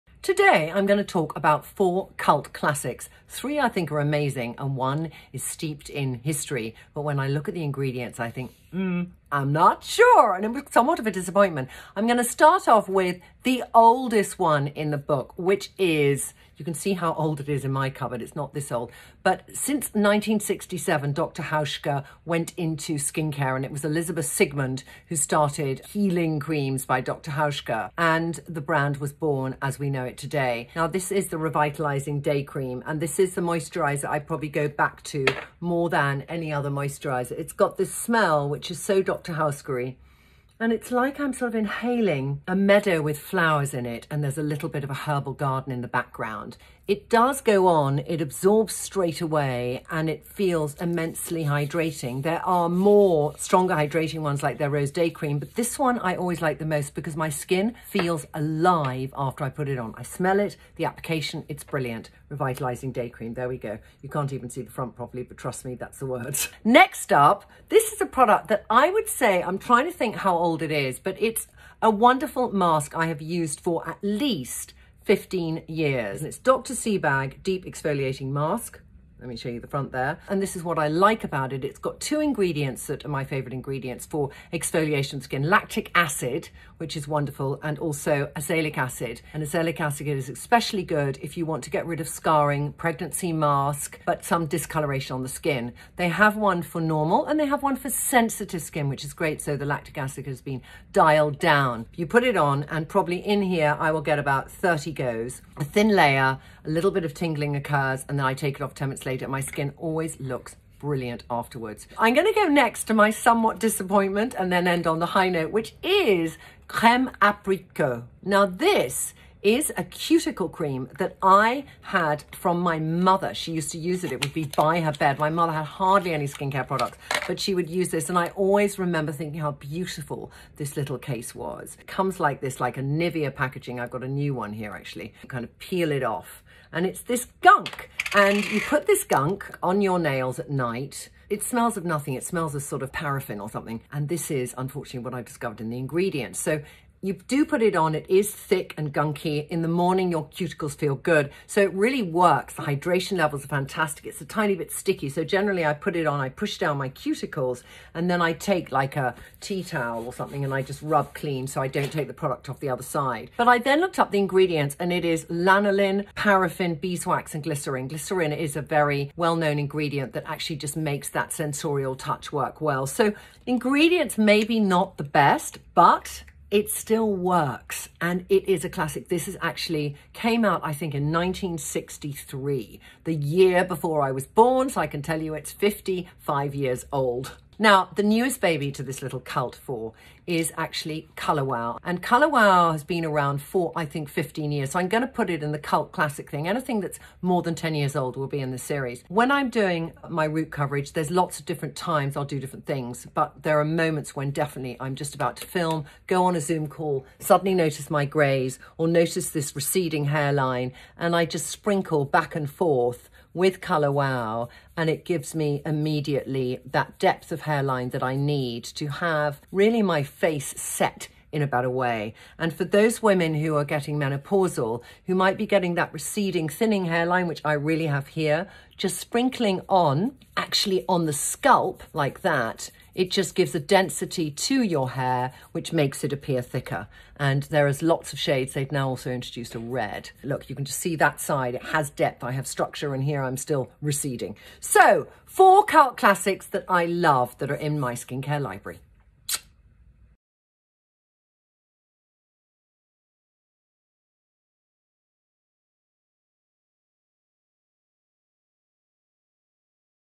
Trinny's Cult Classic Products: Creams, Masks and Root Cover
Join Trinny Woodall as she shares this week’s Cult Classics, including a moisturiser that makes you feel like you’re in a floral meadow, an exfoliating face mask, a cuticle cream my mother swore by, and a root cover-up that helps give the appearance of a fuller hairline…
Revitalising Day Cream by Dr Hauschka
Deep Exfoliating Mask by Dr Sebagh
Creme Apricot by Dior
Root Cover Up by ColourWow